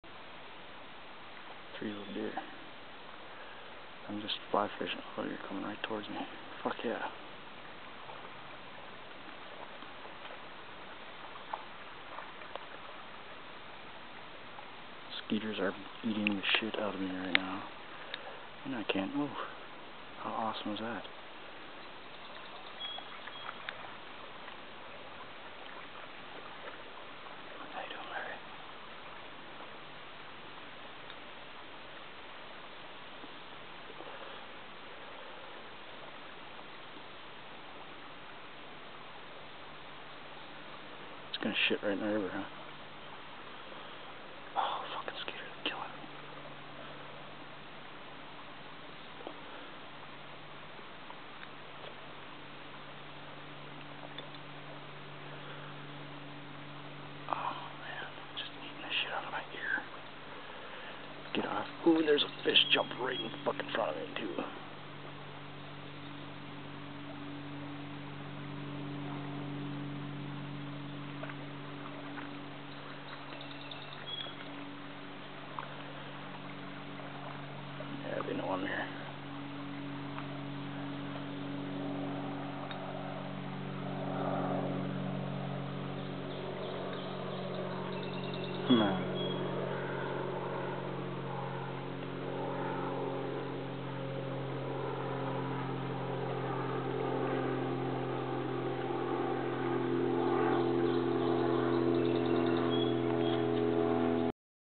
This is why I do the things I do
I was flyfishing in a small stream when 3 yearing deer came out to drink.  I managed to fish out my camera without alarming them and began to film as they came towards me to less than 30 yds. I was in the middle of the stream and the mosquitos were eating me alive but didn't want to spook deer so I froze.  Later a trout surfaced about 6 ft. in front of me.  Too bad I was zoomed in on the deer, woulda been cool to catch that in the frame.
Things like this happen everytime I'm in the woods or on the water and it will never get old.
Sorry 'bout the rough language but the skeeters were tearing me up!!!